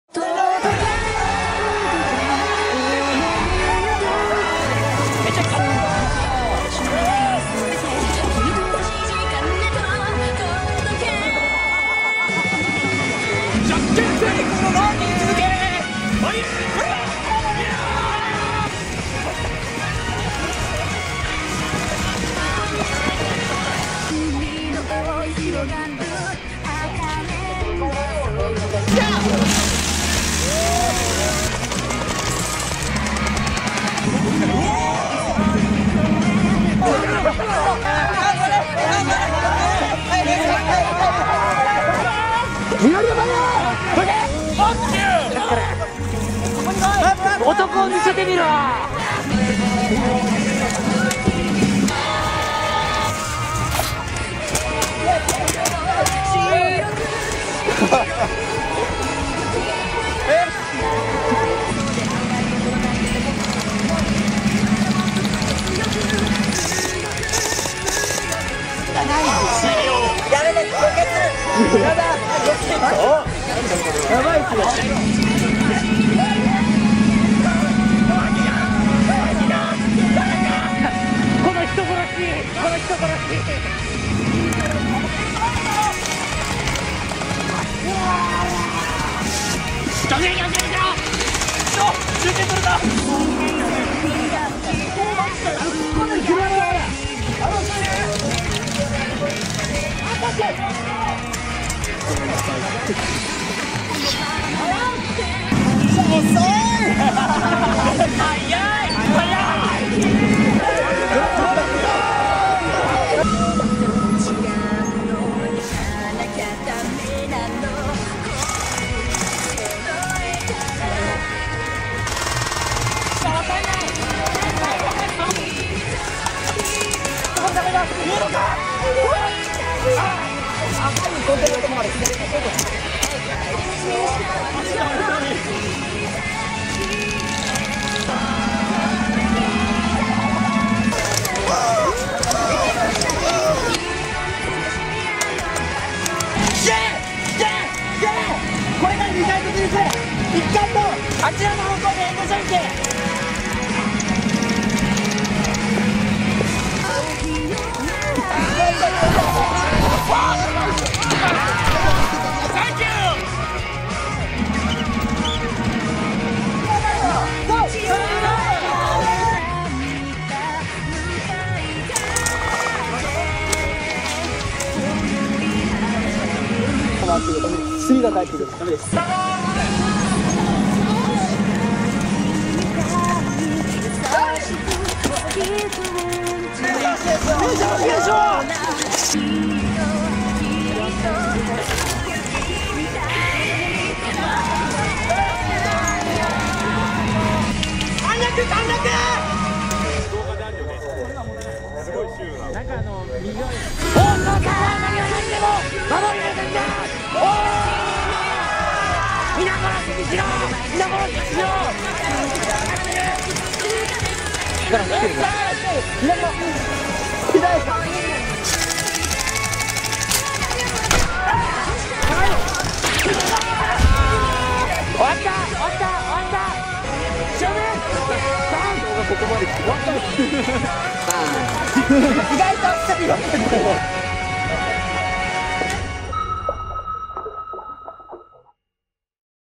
艦隊これくしょんオンリーサバゲ『提督サバゲ 改』秋 本編
雨の中、沢山の艦息に囲まれて、頭の中の一番やわらかい部分に何かが入ってくるような、久々に魂が震えたサバゲでした。正直ゲーム後艦これにド嵌りしたので、次回必ず艦娘で行きますんで、是非声かけて下さい～また遊びまショー
（前回はそうでもなかったんだけどなぁ～やはり艦息効果！？）

【艦これ】提督サバゲ 改 主催 ＠Swallow__k
2015/10/11 千葉県デザートユニオン